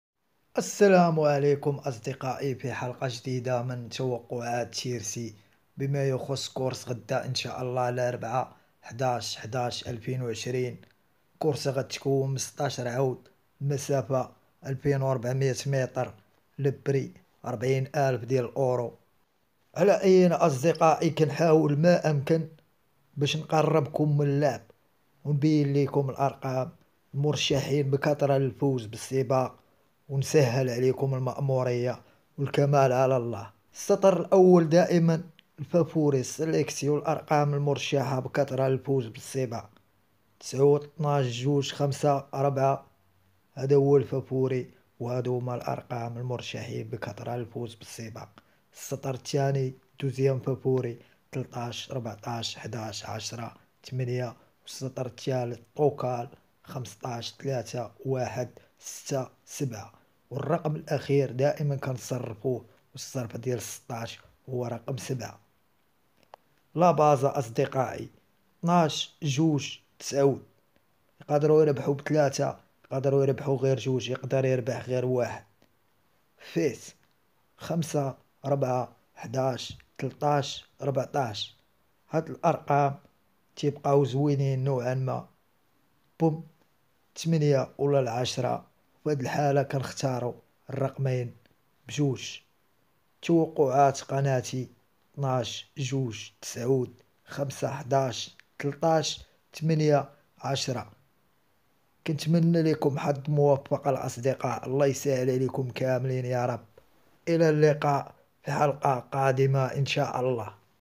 PRIX DE LA VILLE DE TOULOUSE - Plat 11 November PMU Quintè
Mercredi 11 Novembre à 15h15

Quinté+ à Toulouse, Réunion 1, Course n°3, départ à 15h15

PRIX DE LA VILLE DE TOULOUSE - Plat - 2 400 m - 40 000 €

Bon coup du jour !